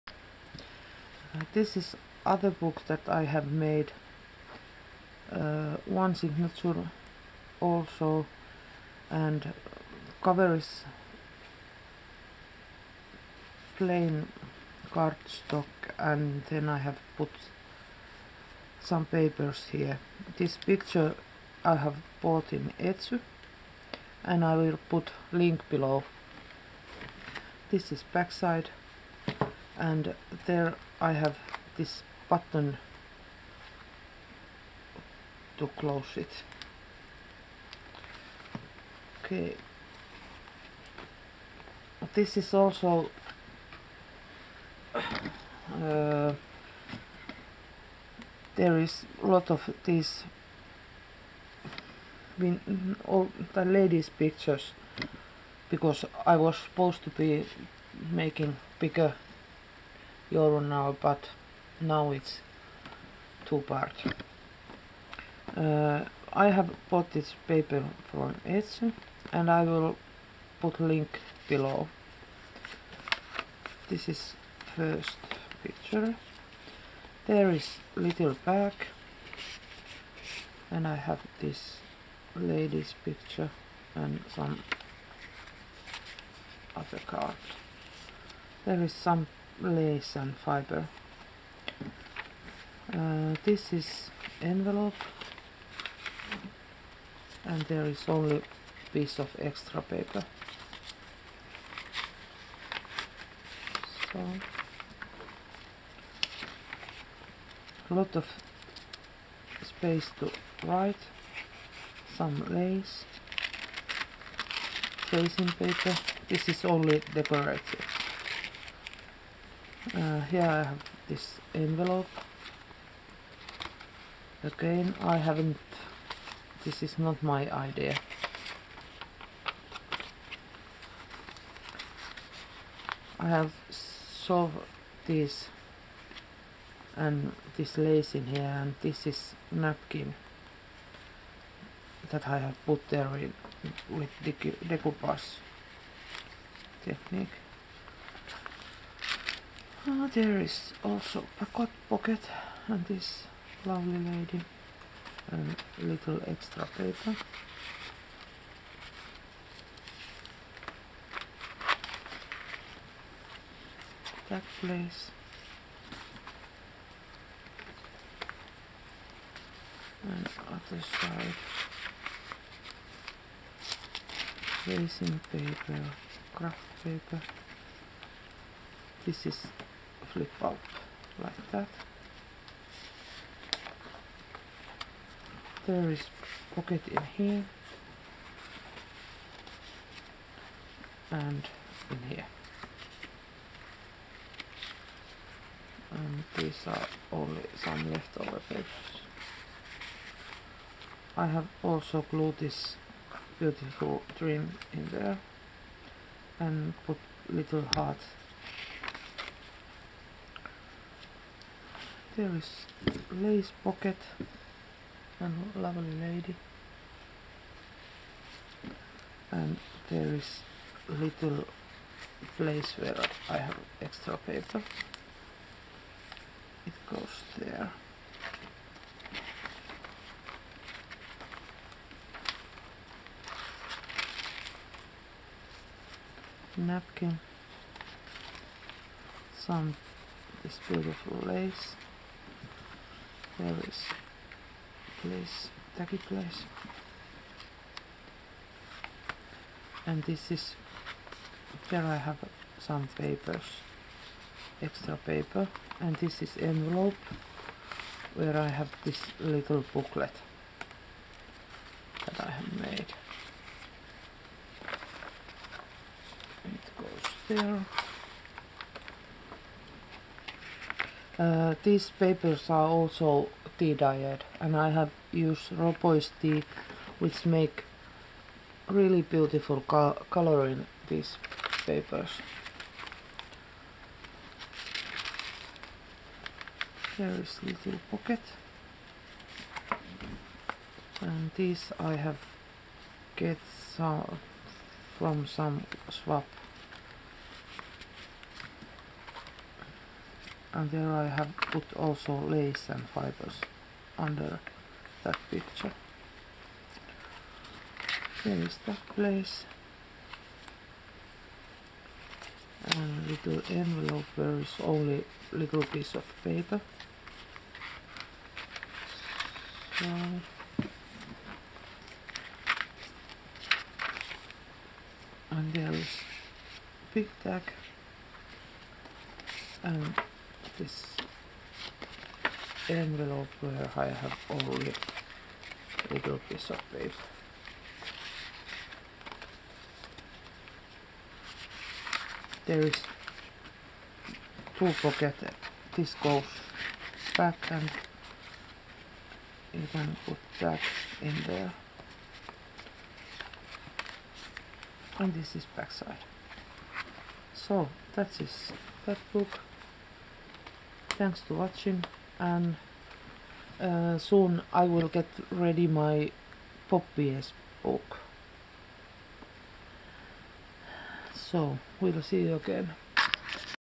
Other vintage journal (sold)
This is second vintage journal, same style than my earlier. Down is link to Etsy shops where I have buy some papers in journal.
Tässä seuraava journal joka on saman tyylinen ku edellinen. Alla linnki Etsy kauppoihin joista olen ostanut osan papereista.
Tämä journal on myynnissä myös mun etsy kaupassa jos olet kiinnostunut. Linkki alla.
My etsy store